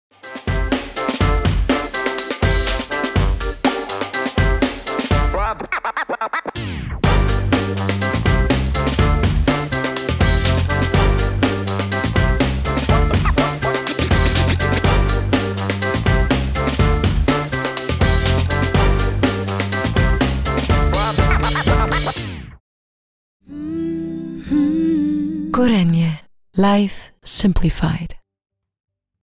Функция FastPreheat - разогрев до 200°С за 5 минут
Функция Fast Preheat разогреет духовку до 200°С всего лишь за 5 минут, что на 30% быстрее прежнего.